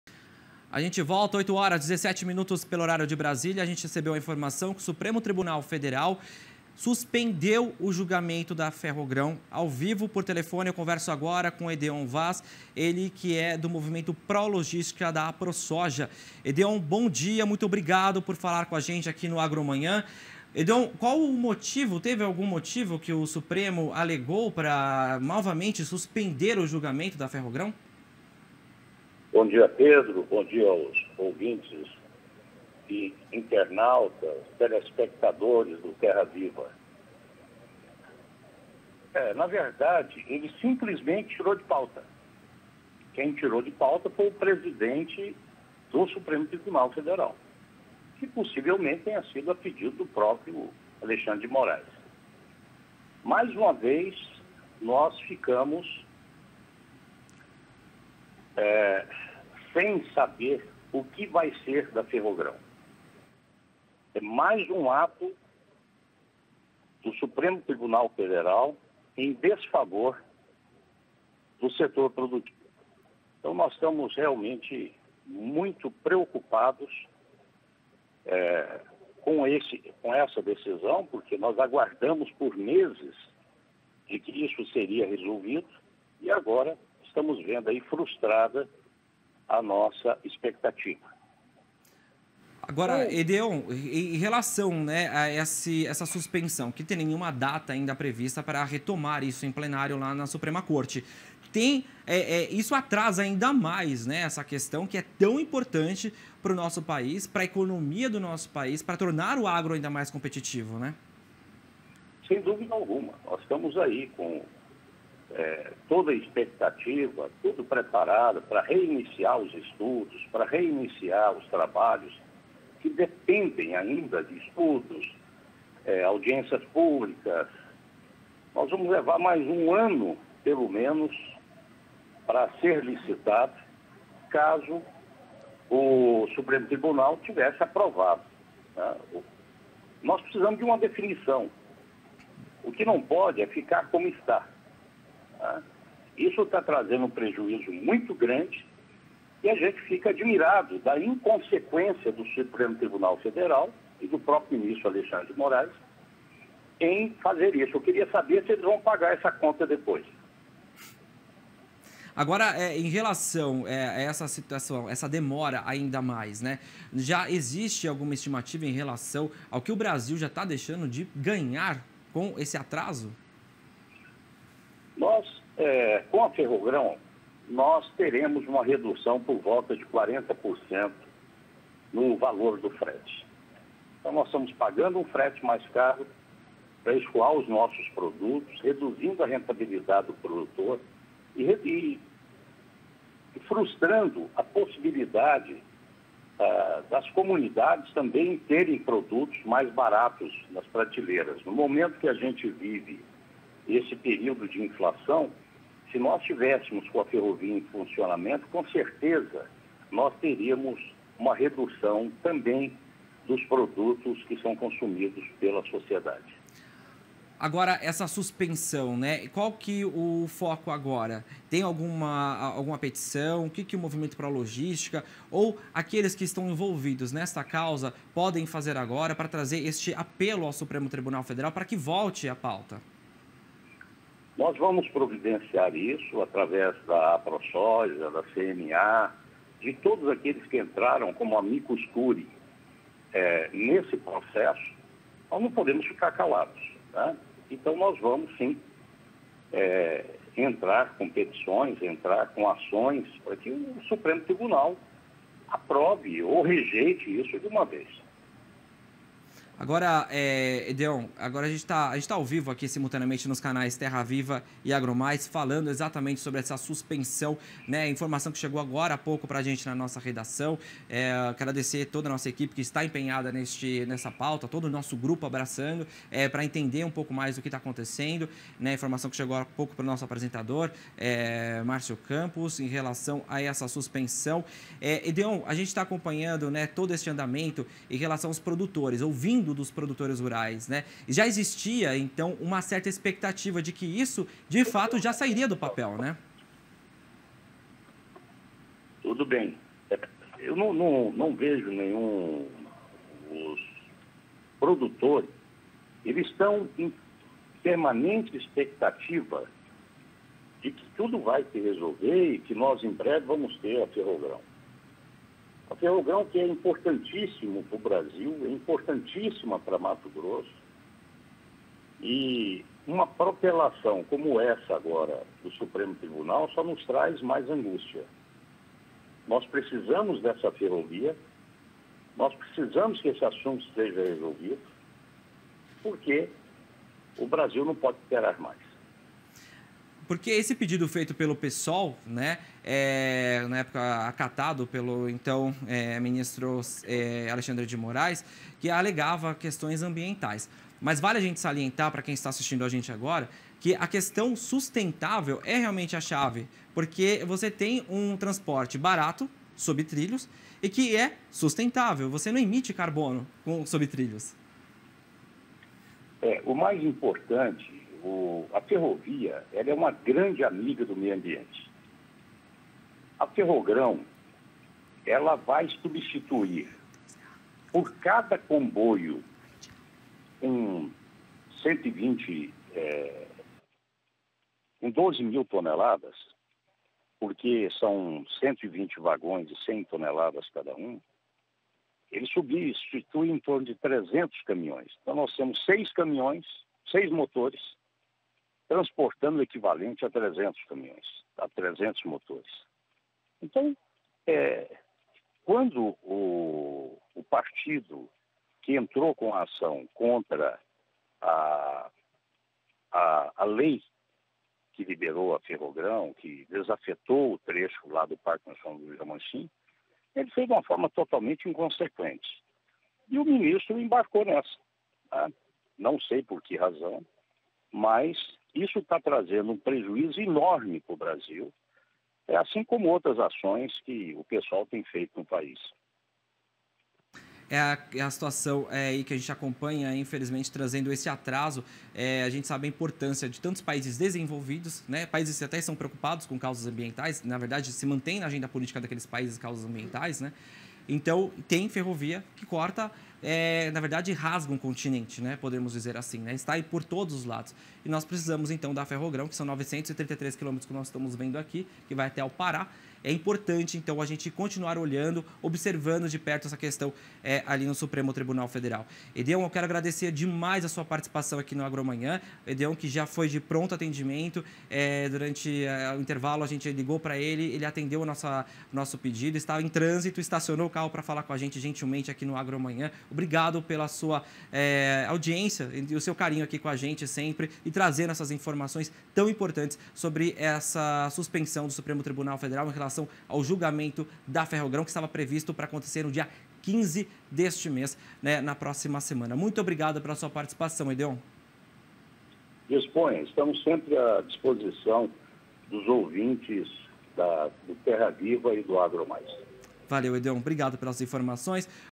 STF suspende julgamento da Ferrogrão
No AgroManhã desta sexta-feira (10), o jornalista Pedro Costa conversou com o representante da Movimento Pró-Logistica MT, Edeon Vaz Ferreira. Eles conversaram sobre Supremo Tribunal Federal ter retirado o julgamento da Ferrogrão de pauta.